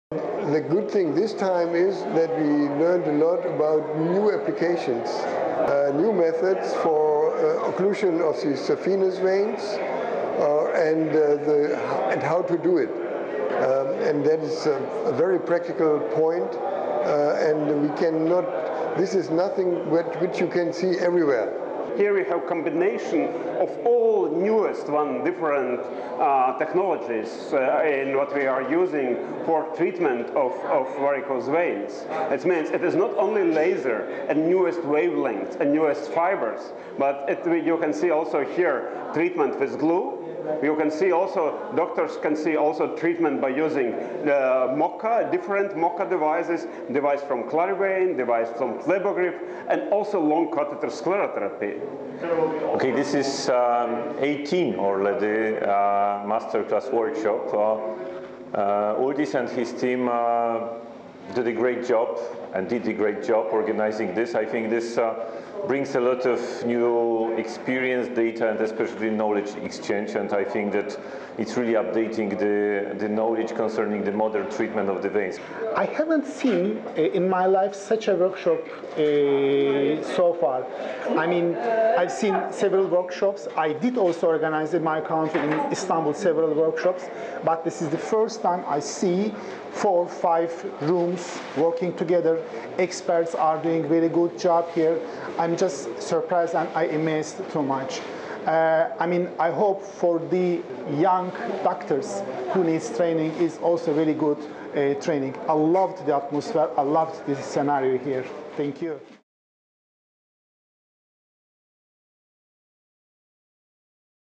18TH INTERNATIONAL MASTER COURSE IN PHLEBOLOGY
On behalf of the Baltic Society of Phlebology I have the honour to invite you to Riga for the 18th International Master Course of Phlebology, that will take place in Riga from 14 – 15 December, 2018.
The main objective of the Course in question is to review and compare the most recent and progressive thermal and non thermal methods for the treatment of saphenous veins. The focus of the Course will be mainly placed on non thermal ab- lations methods and their comparison with actual golden standard - thermal ablation. Additionally, throughout the Course a significant attention will be devoted for a thorough discussion of the best possible functional and aesthetic result with minimal risk of complications and further side effects by using different techniques.

The Baltic Society of Phlebology are delighted and honoured that the faculty of the 18th International Master Course of Phle- bology will be composed of high-level professionals acknowledged all over the world, such as the Prof. Eberhard Rabe, MD (Germany), Prof. Tomasz Urbanek, MD (Poland), Prof. A. Kursat Bozkurt, MD (Turkey), Prof. Felizitas Pannier, MD (Germa- ny), Aleksandra Jaworucka - Kaczorowska, MD (Poland) and Frantisek Zernovicky, MD (Slovakia).

The aforementioned review of the background of the international board of faculty provides the participants with an oppor- tunity to learn about the most successful and up-to-date ablation techniques from the world’s leading physicians specialis- ing in the care of the varicose veins.
The two-day workshop will focus on the latest techniques: non thermal and thermal, as well as provide “tips and tricks” for the successful treatment of vein diseases.